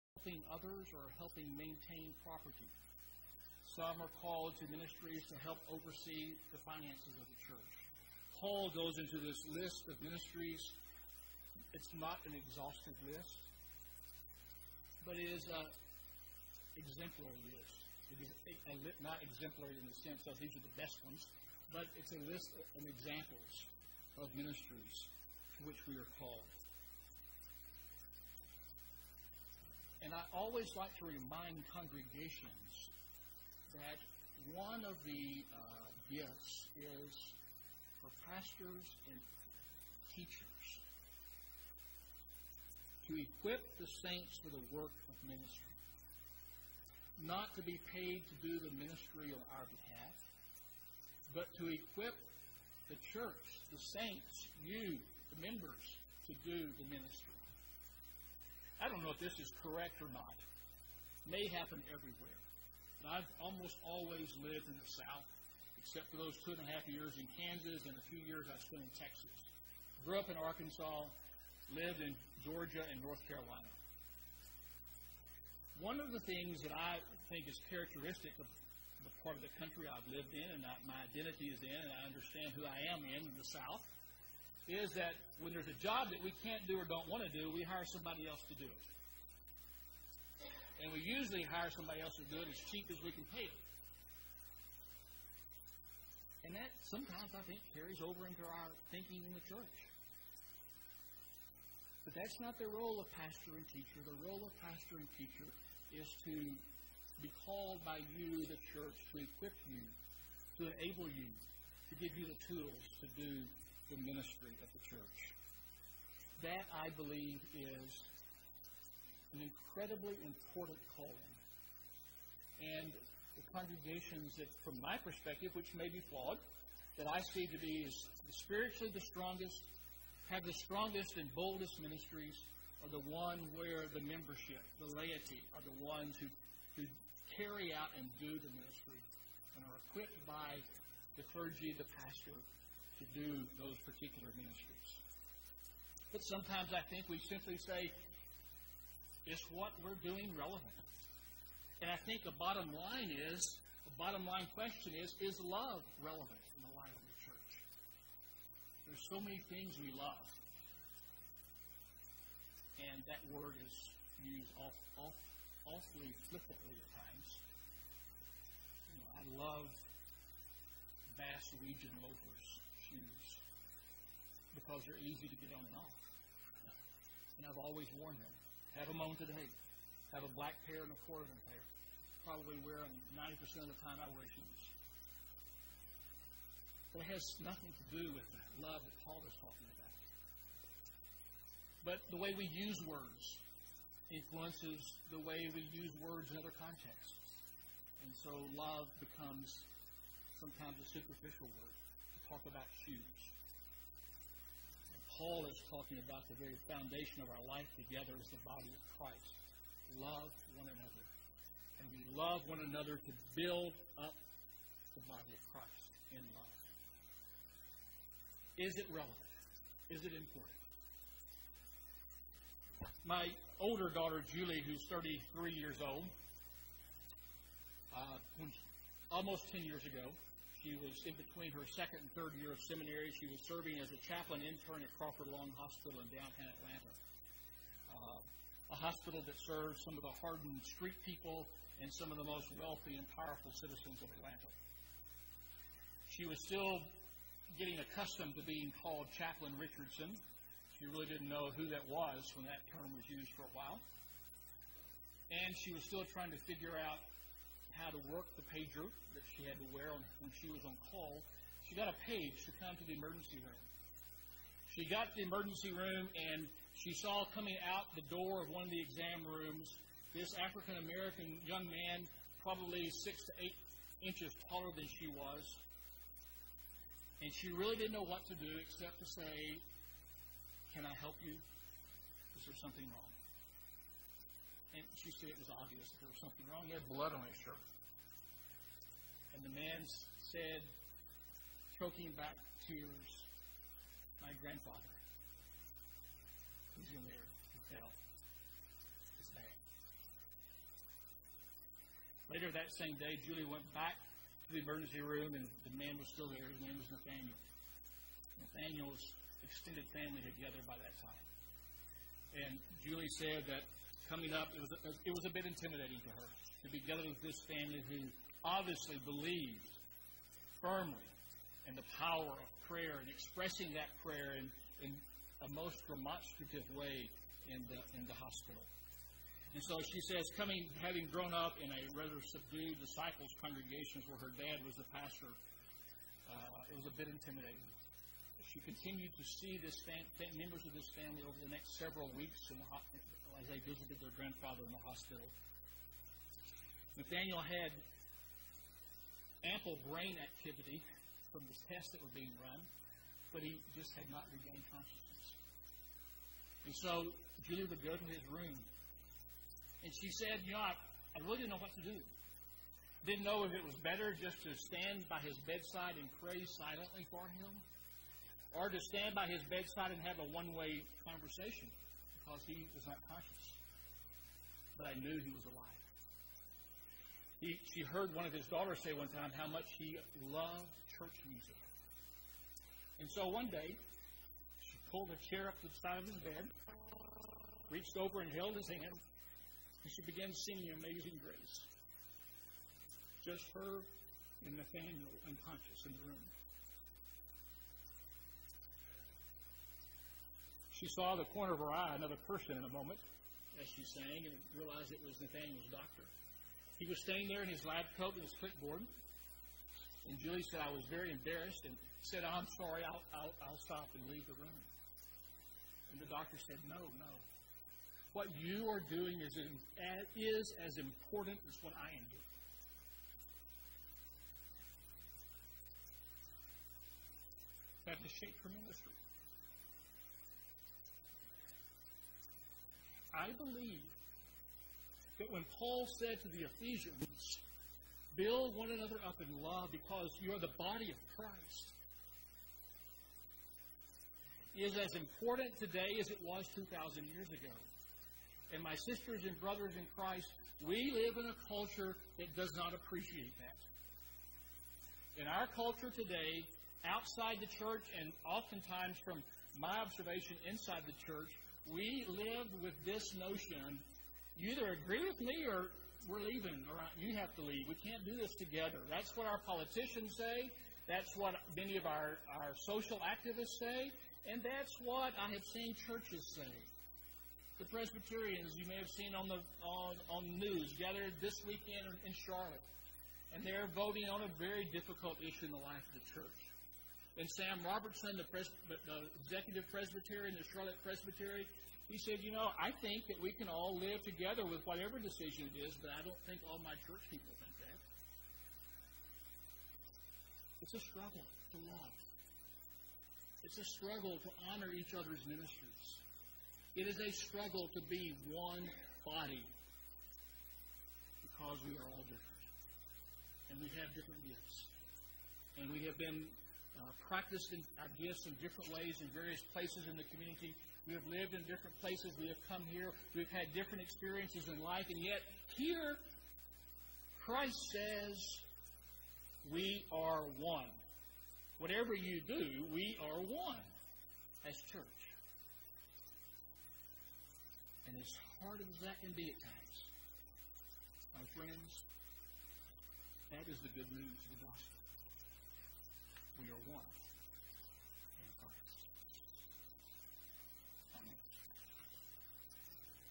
2009 2 15 sermon part 3
John Richardson, Regional Minister speaks about building up one another in love.